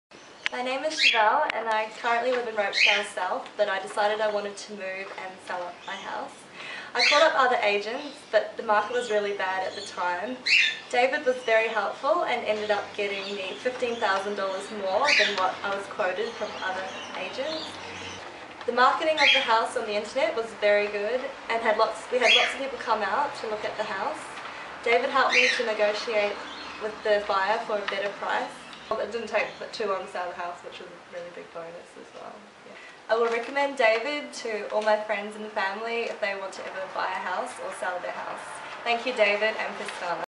Pestana Realty Home Seller Testimonial # 68 - David Ong
I decided I wanted to move and sell my house, I called other agents and even though the market was really bad at the time, David Ong from Pestana Realty was really helpful and he ended up getting me $15k more than the other agents quoted me. The marketing of the house on the internet was really good and we had a lot of people come out to look at the house, David also helped me out to negotiate with the buyer for a better price, also it didn't take too long to sell house which it was a really good bonus as well. I would recommend David to all my friends and family and everyone who would like to sell a house.